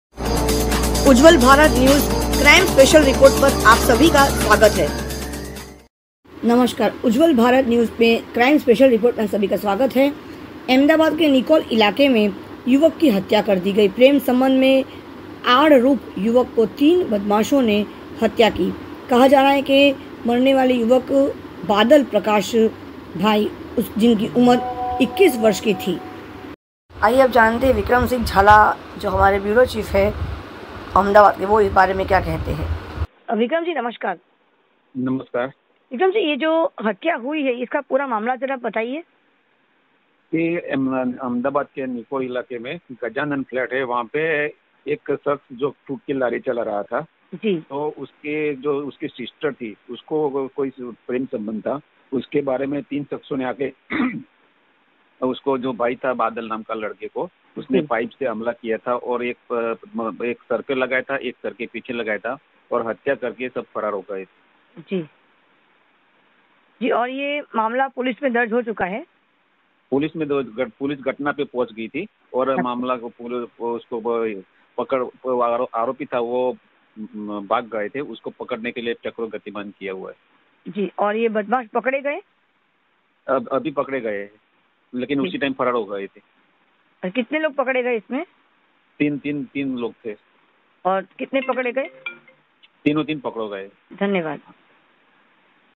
CRIME STORY Special Report
अहमदाबाद के निकोल इलाकेमे युवक की हत्या
प्रेम संबंध में आड़ रूप युवक को तीन बदमाशों ने हत्या की
मरनेबाला युवक बादल प्रकाश भाई उम्र २१ साल

Chief Editor
Seema Bhattacharyya